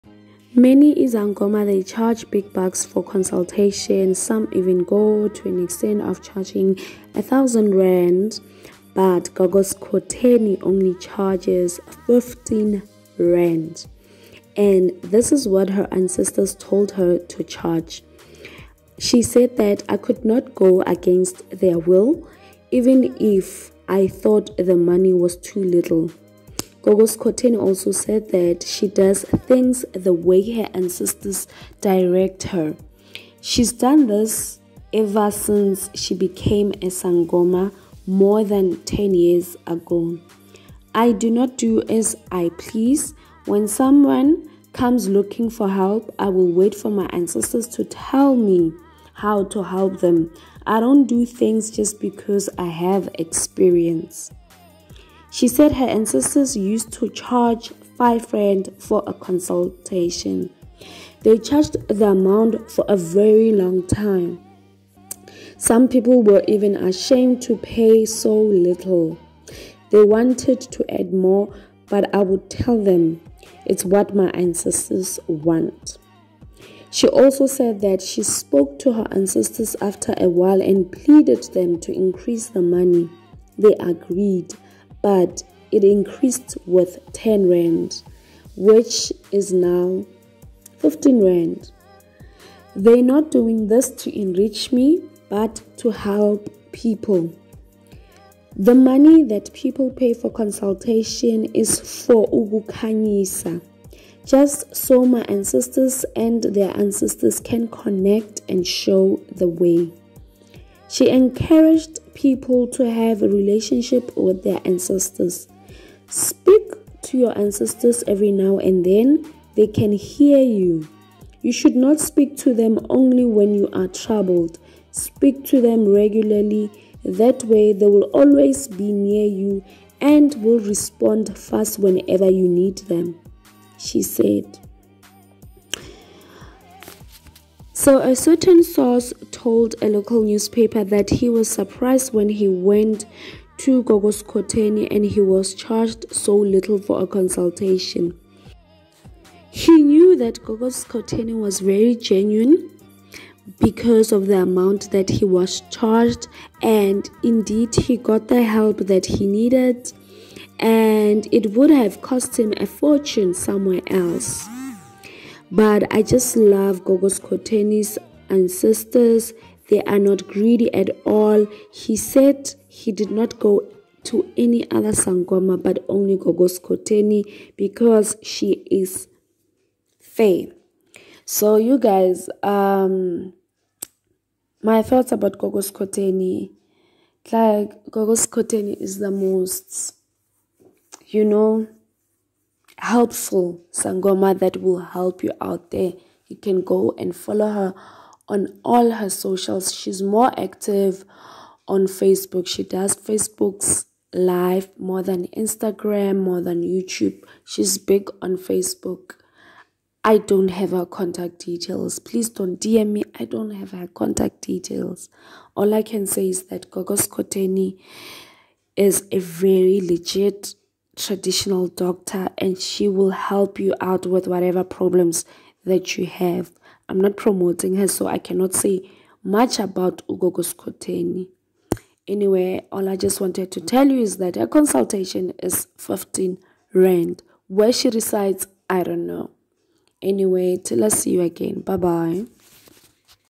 How much Gogo Skhotheni chargers for consultation |South African Sangoma
Gogo Skhotheni is a South African Sangoma that charges little consultation